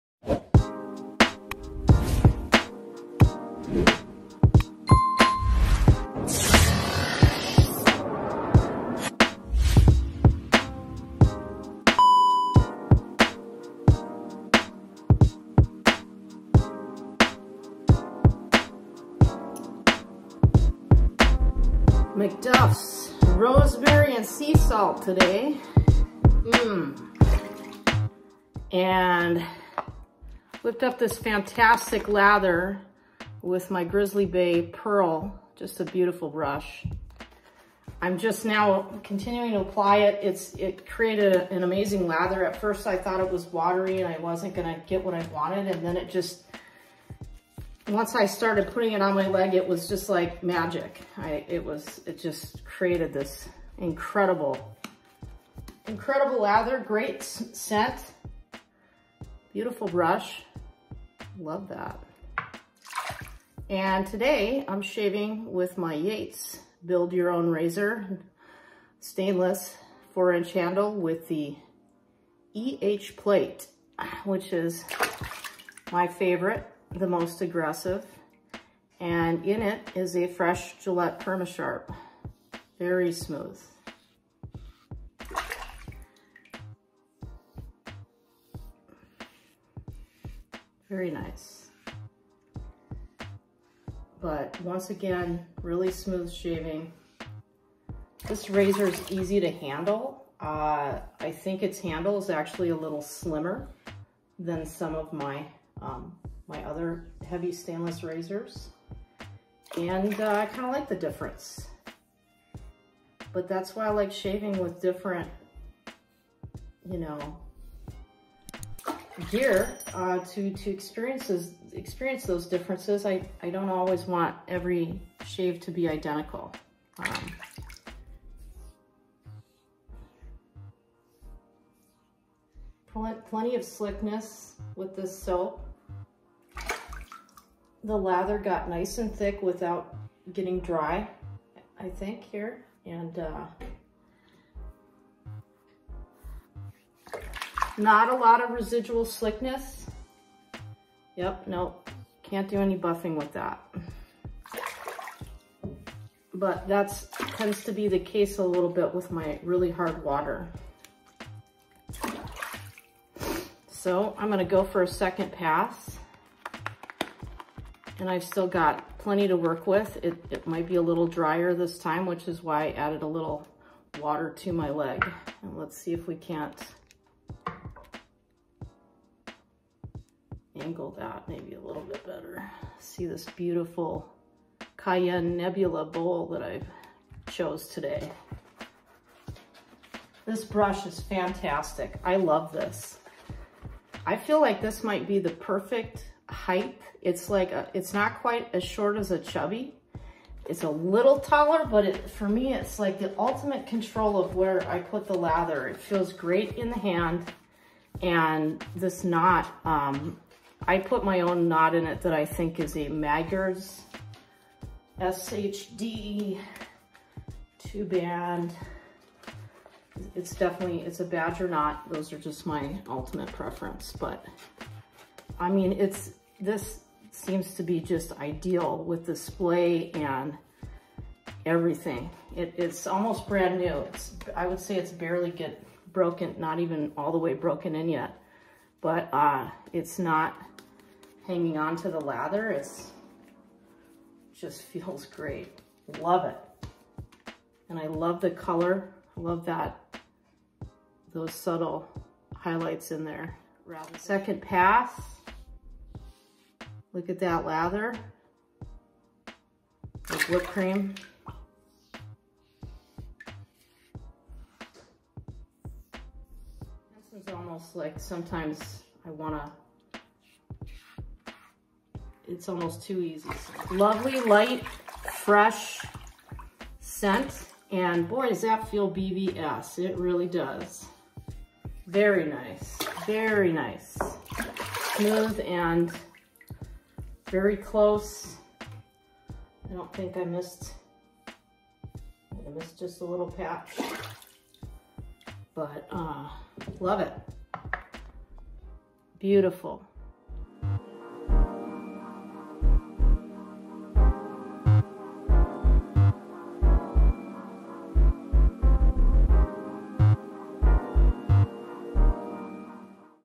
MacDuff's Soap Company Rosemary Sea Salt | Leg Shave
In this video, Katie brings us a leg shave with MacDuffs Soap Company Rosemary & Sea Salt, part of their Explorer Series!

FOLLOW Katie on YouTube: @ladywetshaver